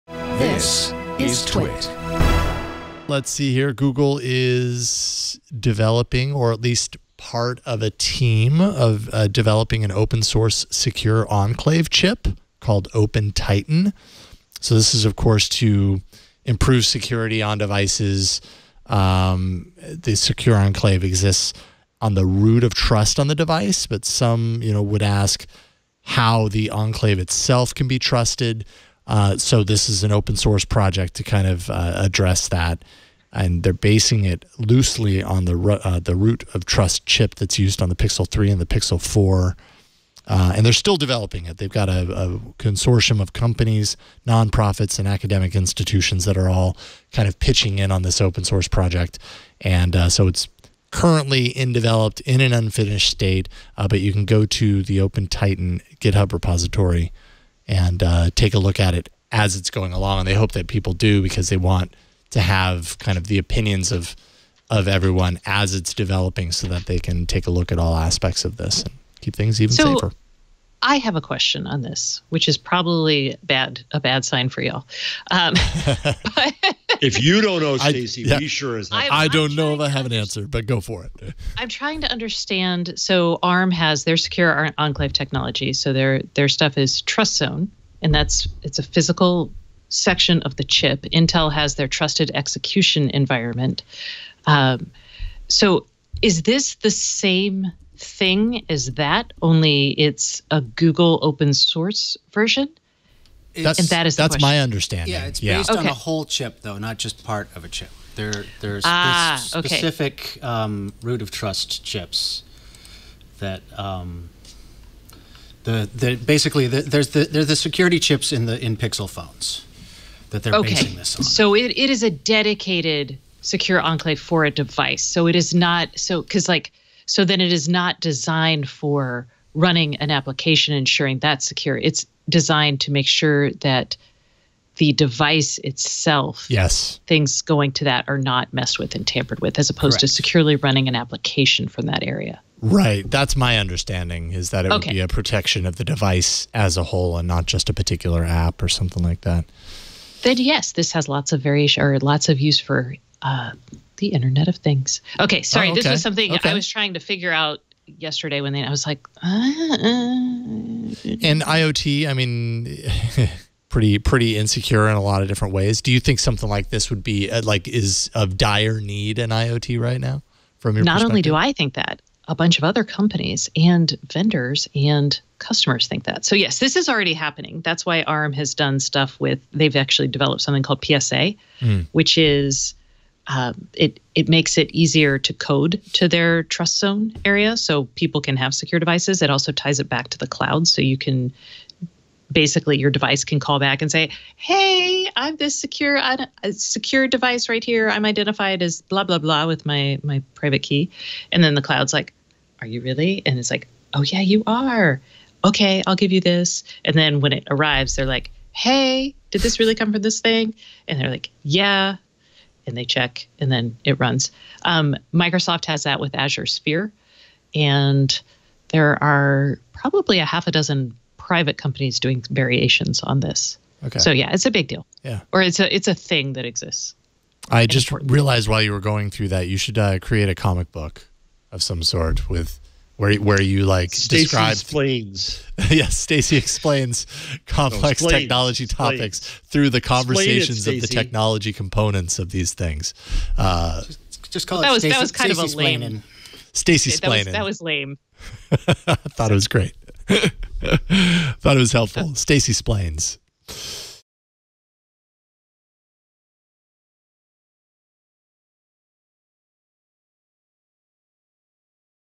Google's open source security chip
Google is working with others to create a secure open source chip based on the security chip in the Pixel 3.